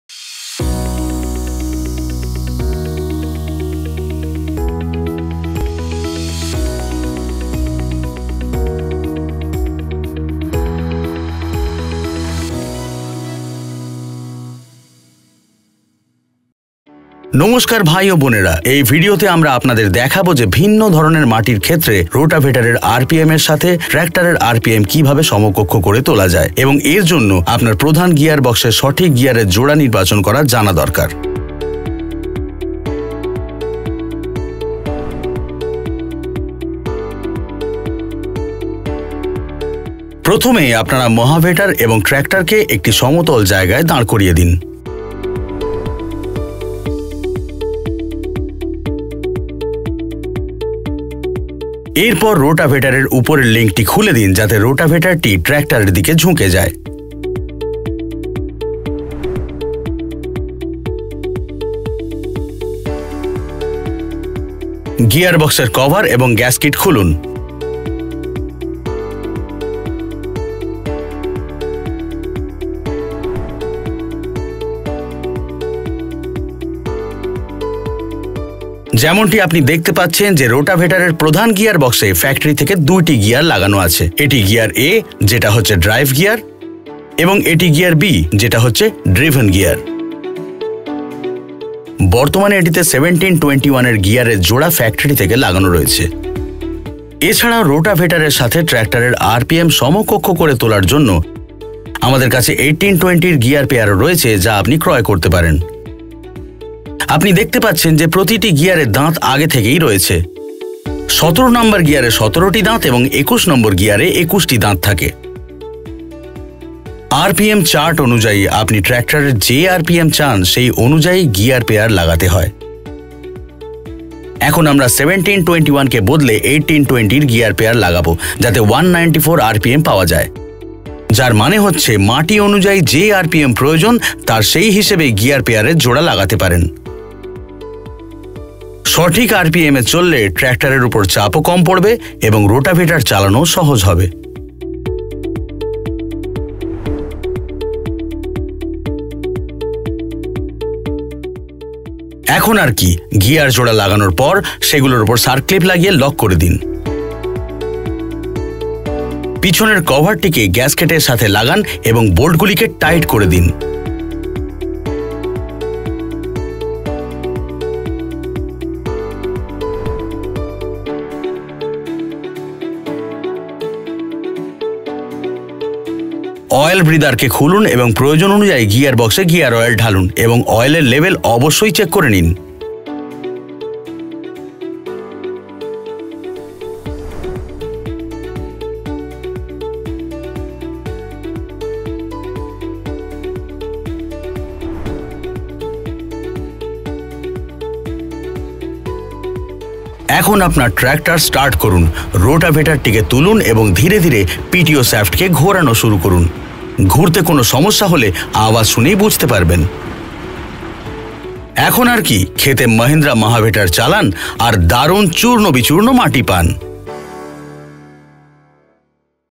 Mahindra | How to change Rotavator gear pair to match Tractor and Rotavator RPM | Bengali
Are you looking for an easy process of changing Rotavator gear pair?
Mahindra presents an effortless process of changing Rotavator gear pair to match Tractor and Rotavator RPM. Different gear pair combinations provide different RPM for Rotavator to work best in various type of soils both in dry and wet soil conditions, ensuring good quality of pulverisation.

Watch this video and experience the effortless change of gear pair combination!